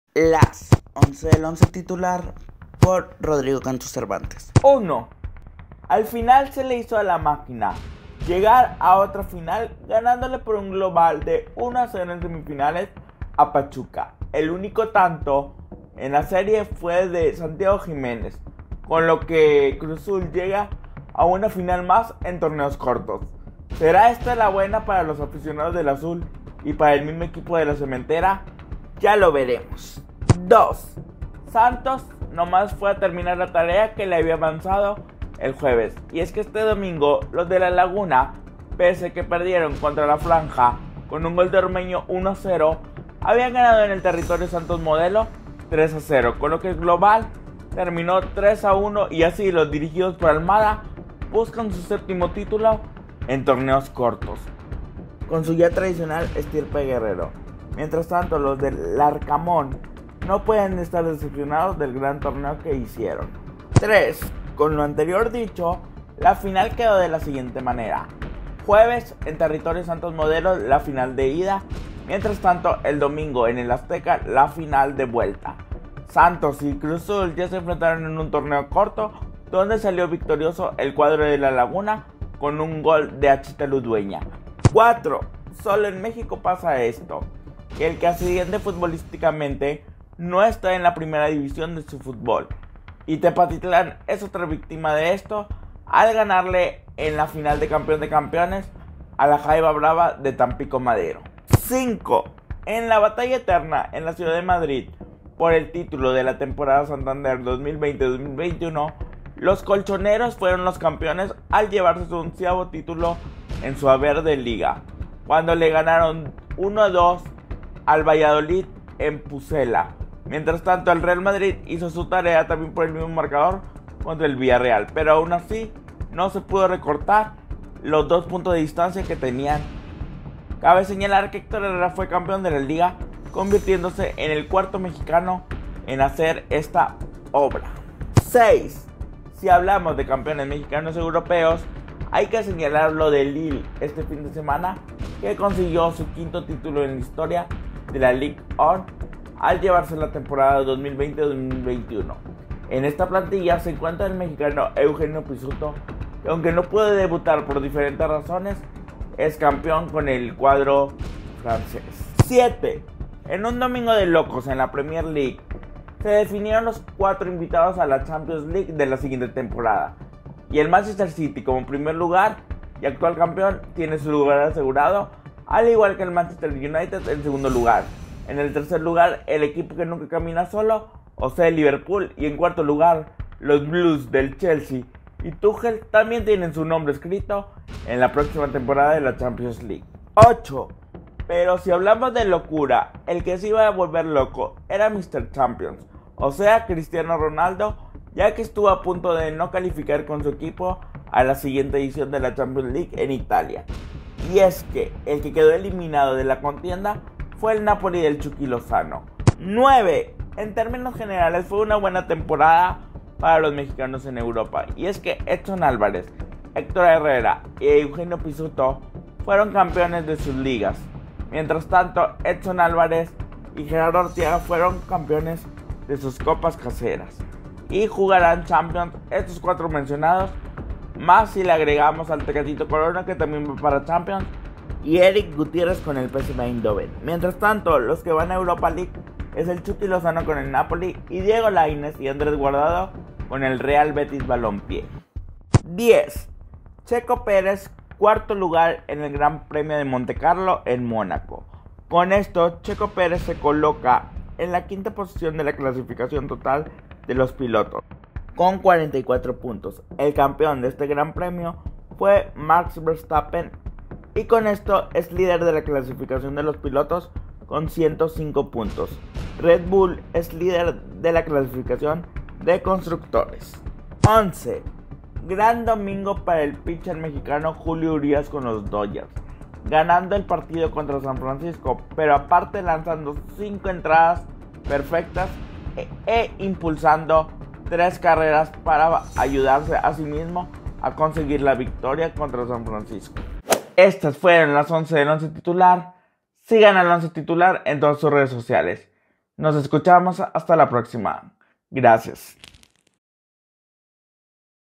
las once de eloncetitular (24MAYO202)
En ModoGuerrero ⚽️⚽️⚽️ FelizLunes con Twitter @rodcace

Justo para el CruzAzul 🚂estar en la FINAL del Guard1anes2021 🇲🇽

Buen torneo para 🎽LaFranjaQueNosUne y también justo 4 lugar para Checo en MonacoGP

¡Juega diferente!

Y recuerda visitarnos en;